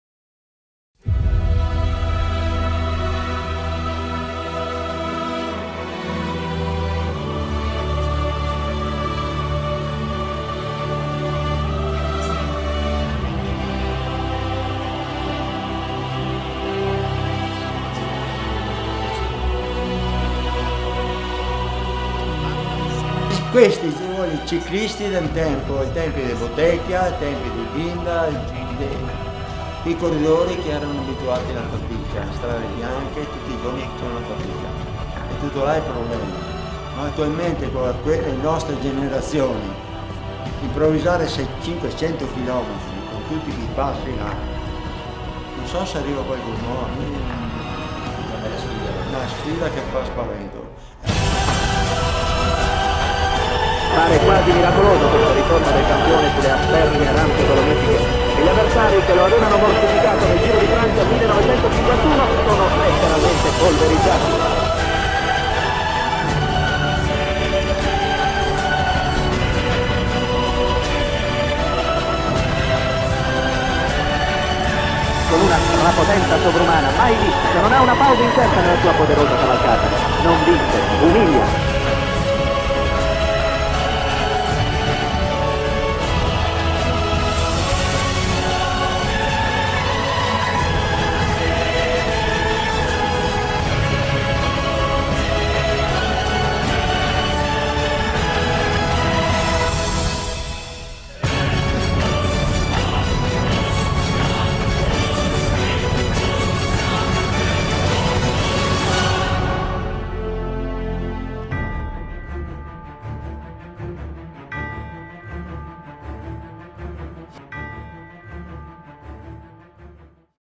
ULTRACYCLING DOLOMITICA - THE CHALLENGE
The most important cycling uphill race of all time, on the most famous mountains of the story of cycling. 580km 15500mt
Let the challenge begin.